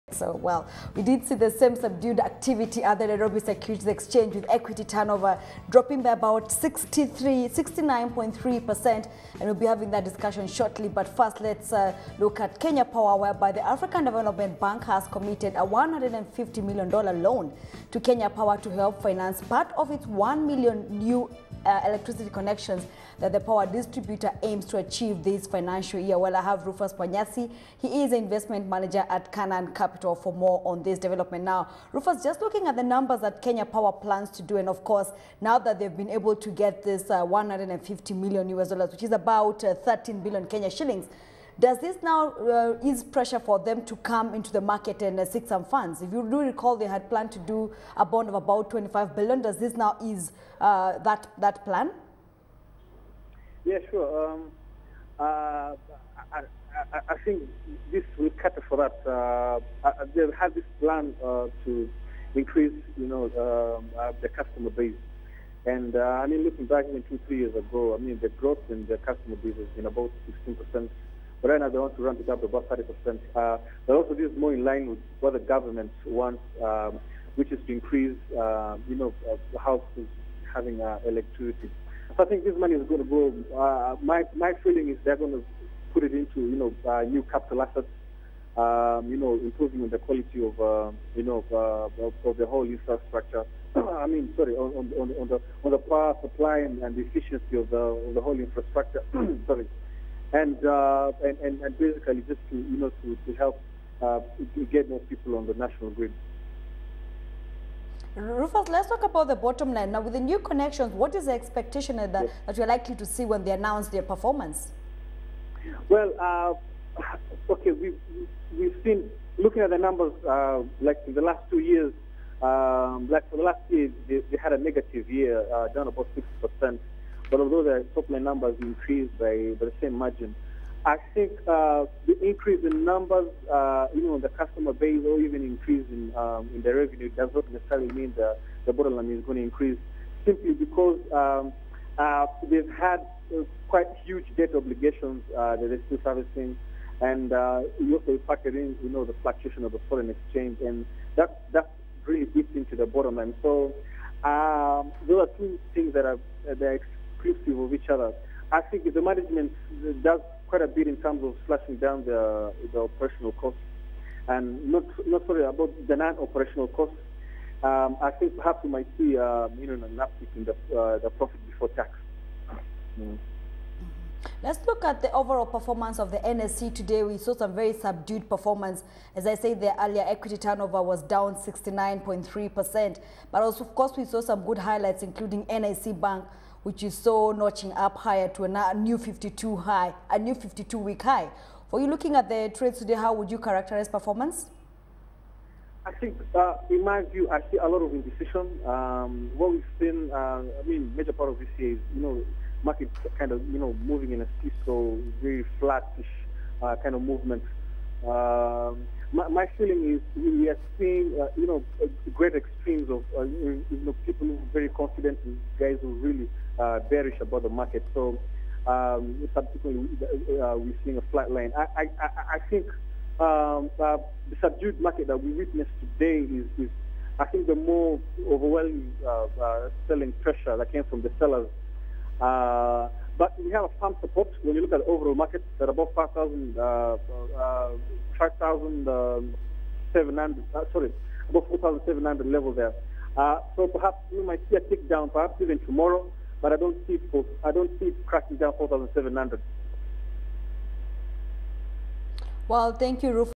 Update on Kenyan markets
Original Publish Date: || Mon, 25 Aug 2014 13:03:47 GMT || Joining CNBC Africa to give an update on the Kenyan markets is Rufus Mwanyasi, investment manager at Canaan Capital.